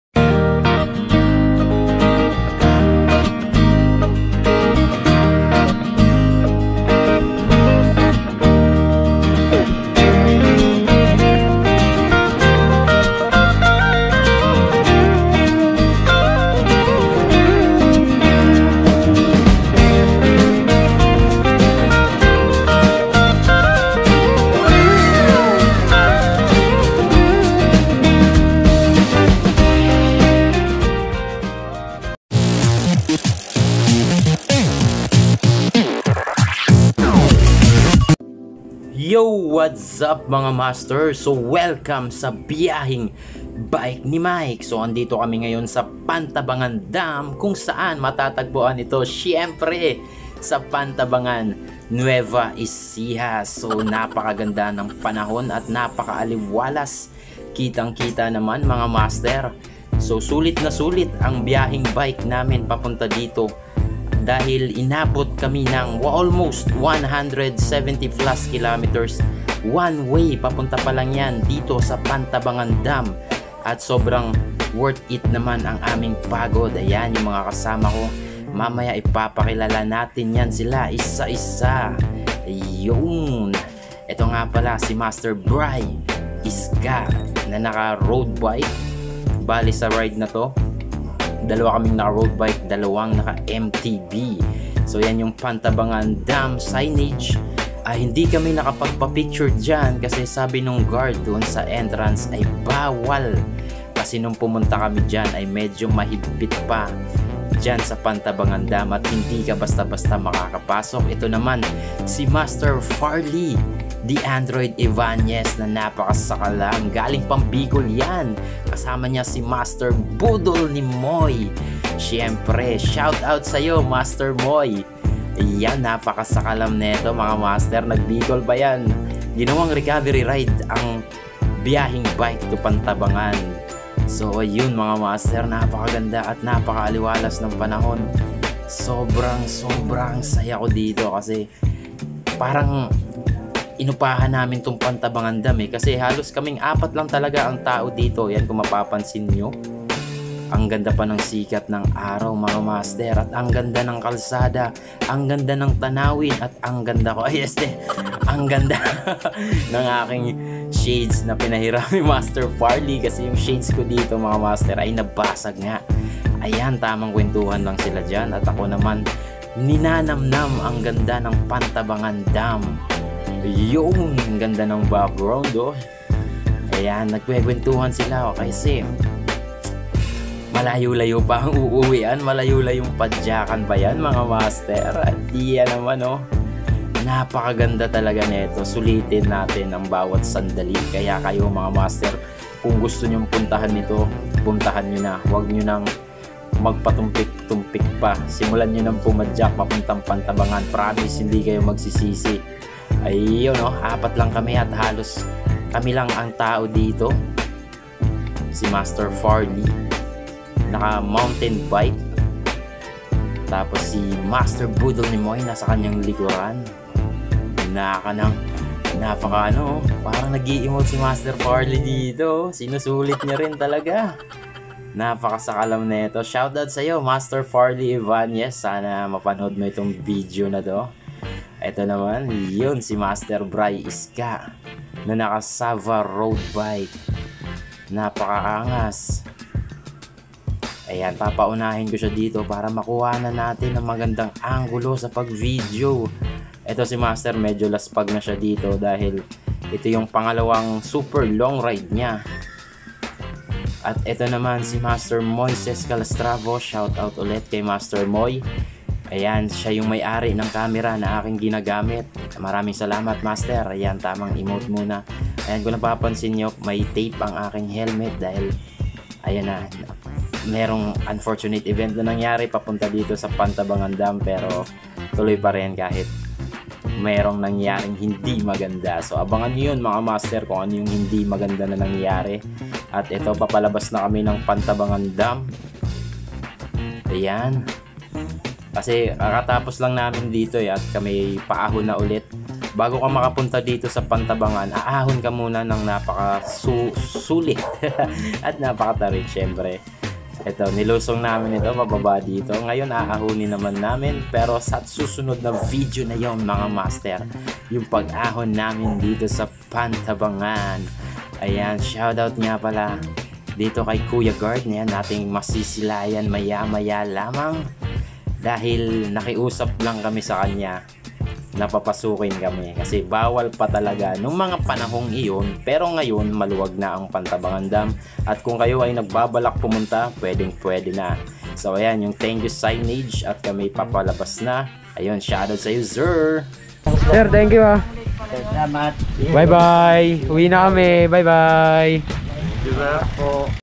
Pantabangan Dam Byaheng Bike
Very relaxing view and sceneries dito sa Pantabangan Dam mga Masters. Isama niyo na eto sa BIKEIT LIST niyo. Sulit na sulit ang pagod.